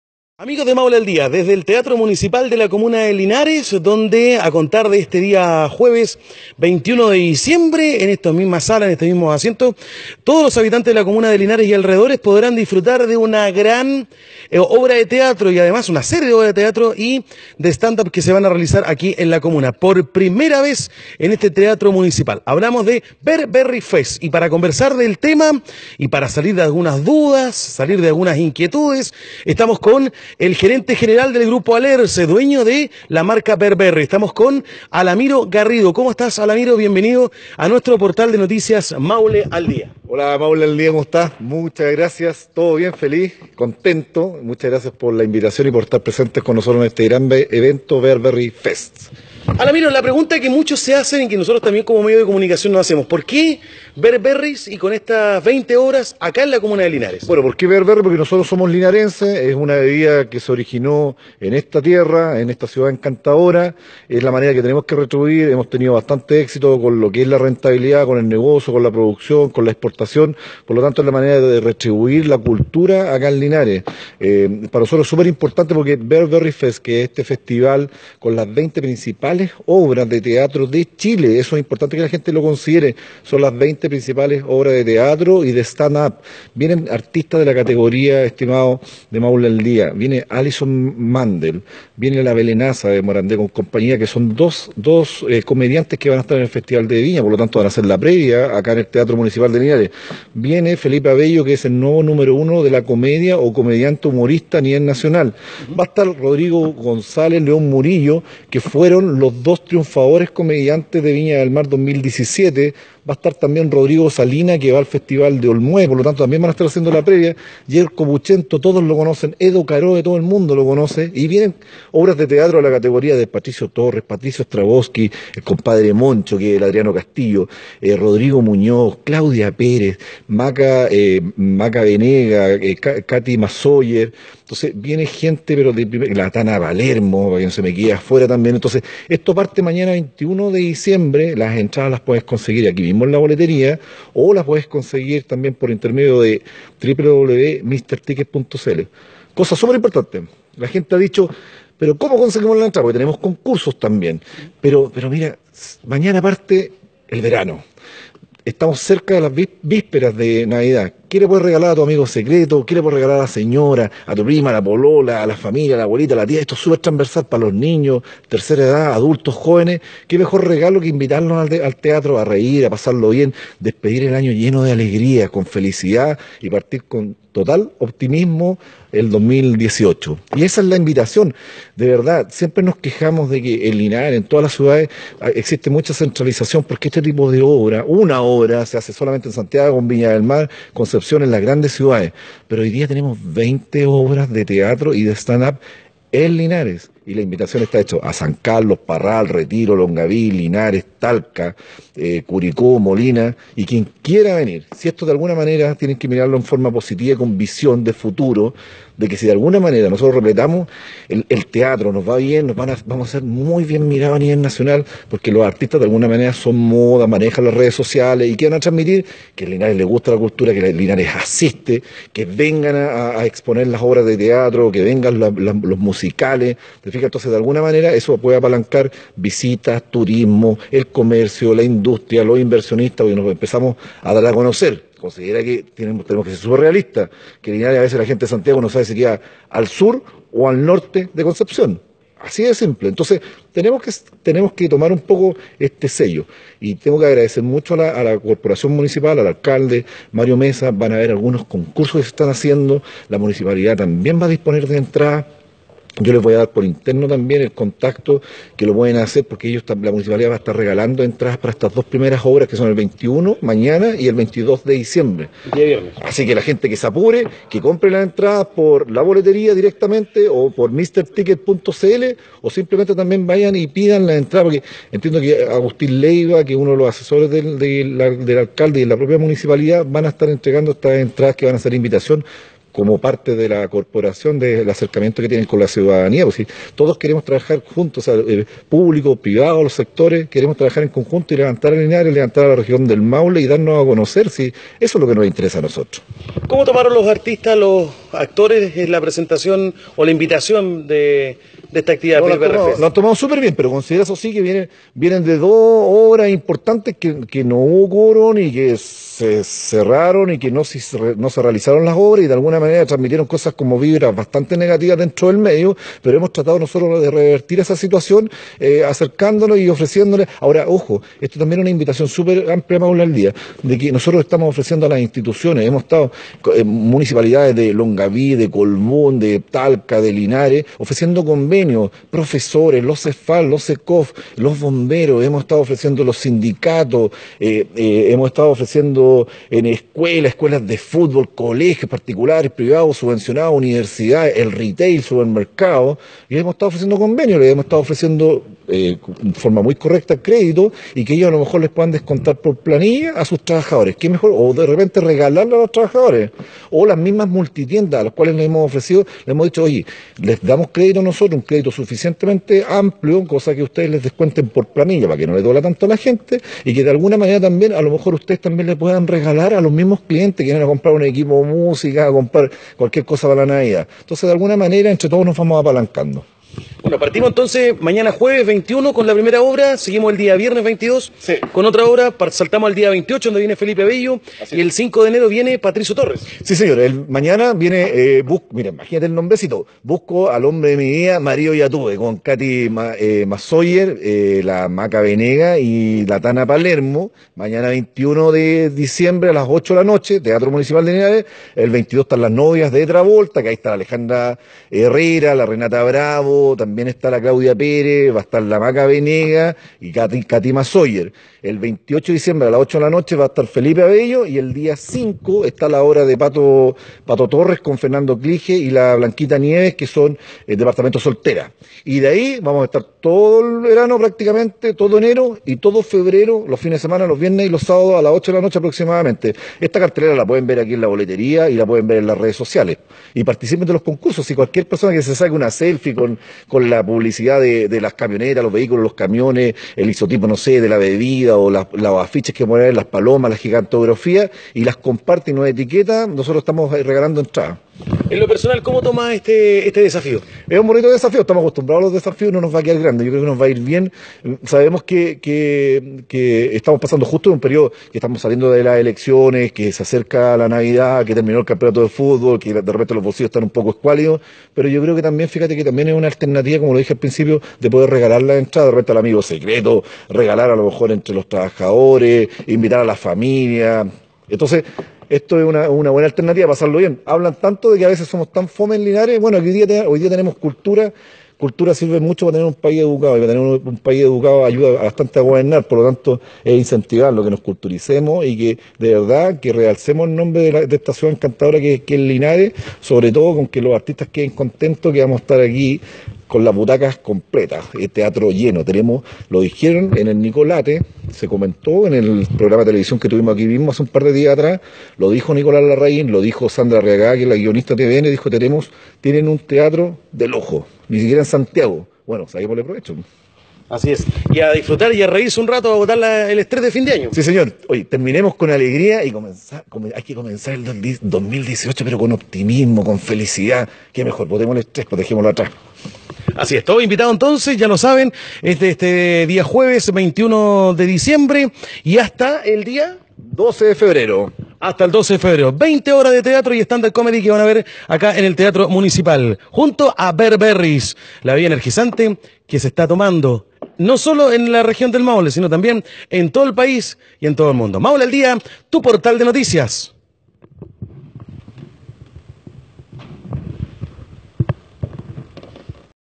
BEAR BERRY NATURAL ENERGY DRINK junto a Don Alamiro Garrido Hablando Con Maule Al Dia
BEAR BERRY NATURAL ENERGY DRINK, Nos Invita a Festival Bear Berry Fest.
Primer Festival de Teatro de Linares y Stand Up.
Desde 21 de Diciembre 2017 a  11 de Febrero 2018.